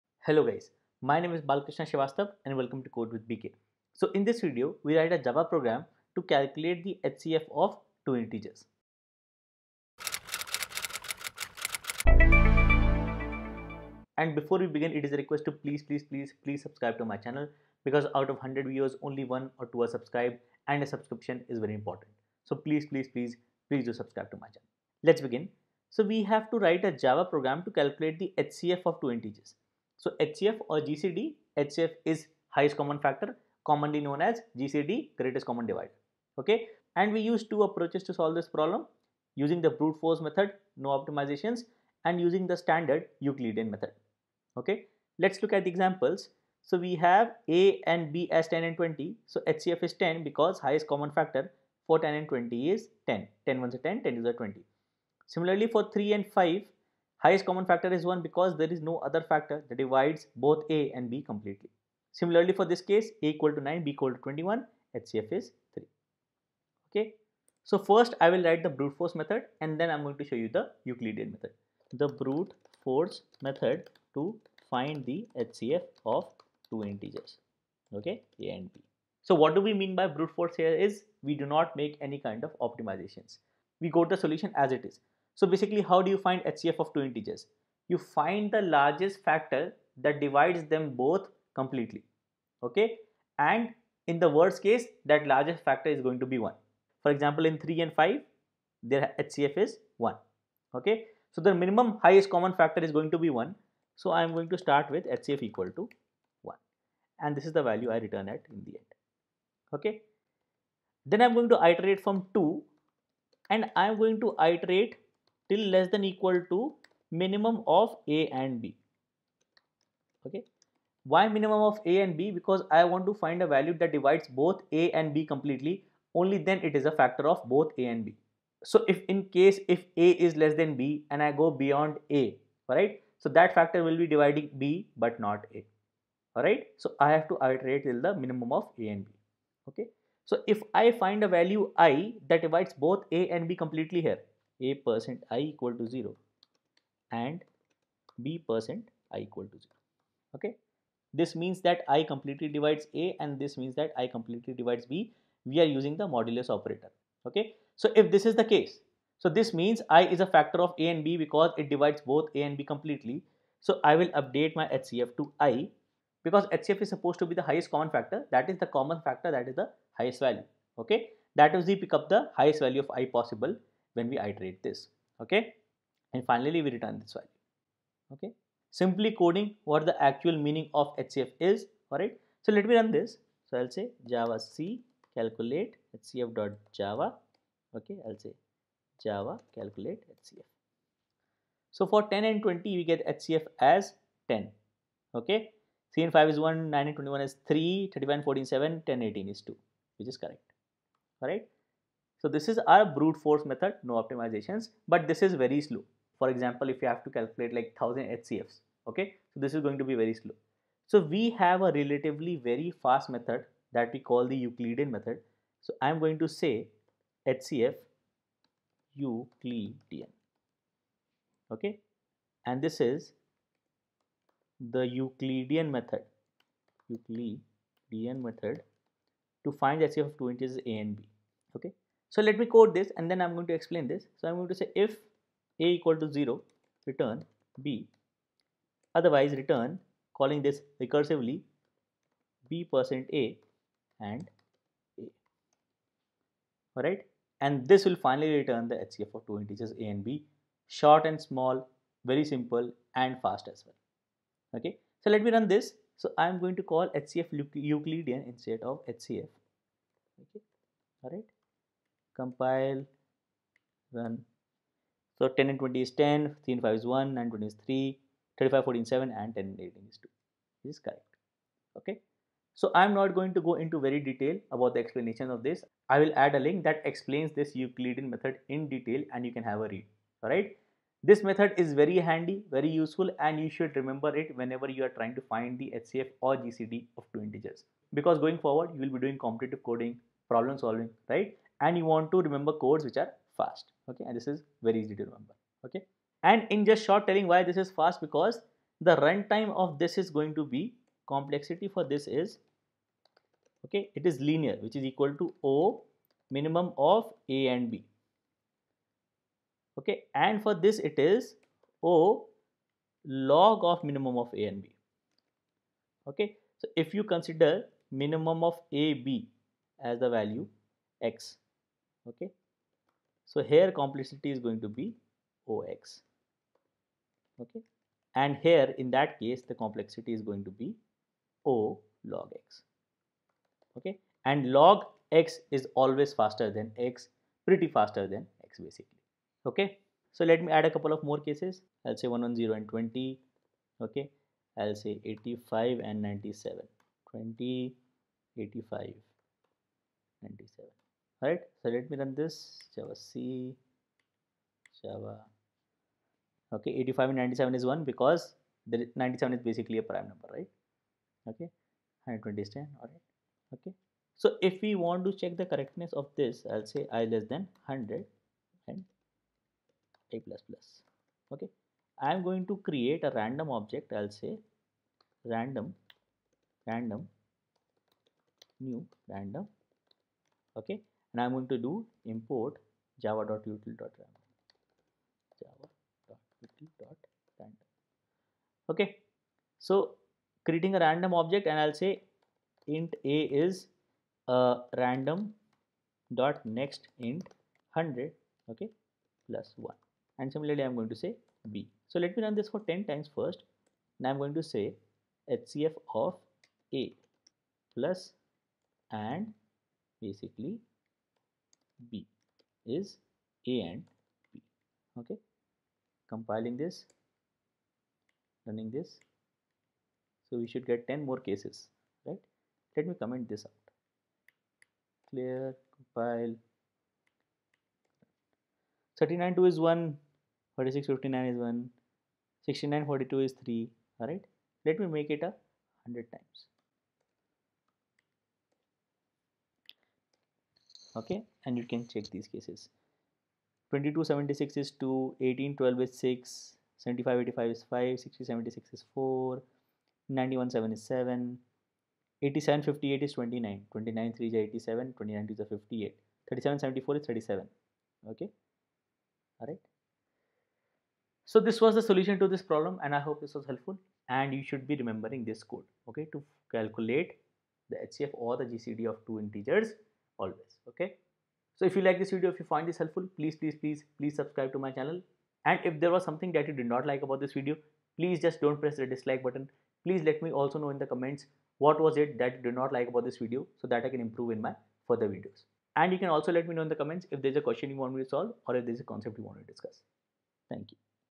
Java Program to Find HCF (GCD) of Two Numbers
Welcome to CodeWithBK — your go-to hub for mastering Java programming.

In this video, we will learn how to write a Java program to find the HCF (Highest Common Factor), also known as the GCD (Greatest Common Divisor), of two numbers. This is a fundamental mathematical problem often used in number theory, programming practice, and competitive coding.

Example 1:
Input:
a = 10, b = 25
Output
5

Example 2:
Input:
a = 3, b = 7
Output
1

In this video, you will:
- Understand what *HCF (GCD)* means and why it is important
- Learn how to calculate the HCF of two numbers using different approaches
- Implement the solution using *loops* and the *Euclidean Algorithm*
- Practice working with *conditional statements, loops, and modular arithmetic*
- Strengthen your problem-solving skills in mathematical programming with Java

Why learn this program?
The *Java program to find HCF (GCD)* builds strong foundations in mathematical logic, number theory, and algorithmic problem solving. The Euclidean Algorithm is one of the most efficient and widely used methods to compute GCD, making it an important concept for programming interviews and competitive exams.

If you want to improve your skills in *mathematics-based Java problems, loops, and algorithms* , this tutorial is perfect for you.

What is the Euclidean method?

Perfect for:
- B.Tech / BCA students
- AP Computer Science students learning Java
- Beginners who want to practice *Java loops* and *conditional logic*
- Students learning *mathematical programs in Java* for exams and assignments
- Developers preparing for *competitive coding challenges*
- Anyone preparing for *Java interview questions* related to number theory

Programming Language: Java
Level: Beginner-friendly

Timestamps:
00:00 Intro
00:25 Problem description
01:07 Java code using brute force approach to find the HCF of two integers
03:30 Java code using Euclidean method to find the HCF of two integers
06:04 Correctness of the code